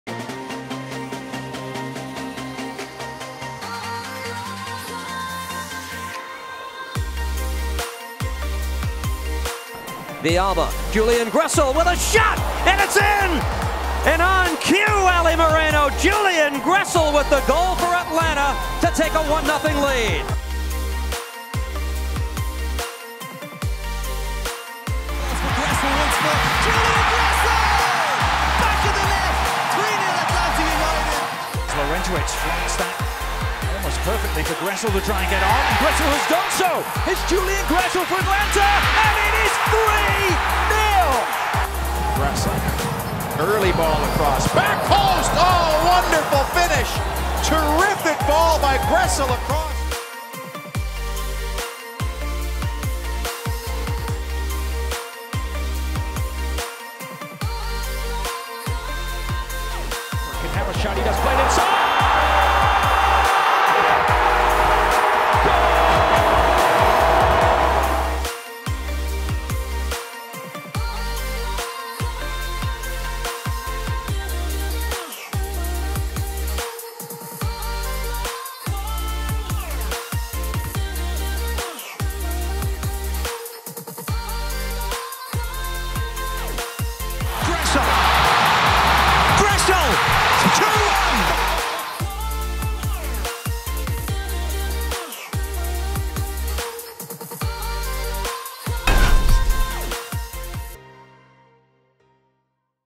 Julian Gressel named MLS AT&T Rookie of the Year
Julian Gressel made an instant impact on Atlanta United in his first year in the league. Here are his best moments.